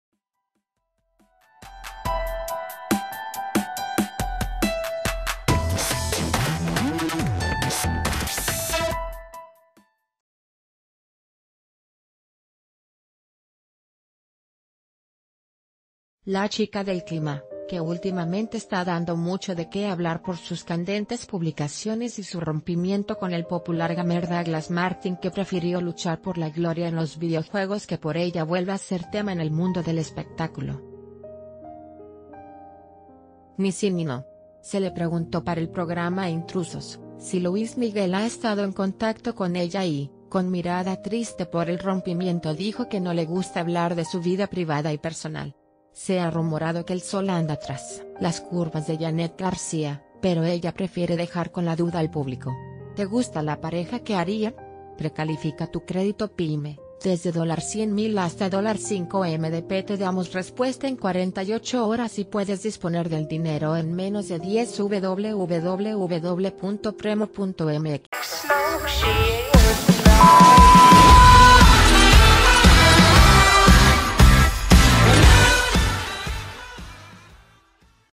Yanet García responde a si hay 'algo' con Luis Miguel
'La chica del clima', que últimamente está dando mucho de qué hablar por sus candentes publicaciones y su rompimiento con el popular 'gamer' Douglas Martin -que prefirió luchar por la gloria en los videojuegos que por ella- vuelve a ser tema en el mundo del espectáculo. Ni sí ni no. Se le preguntó p...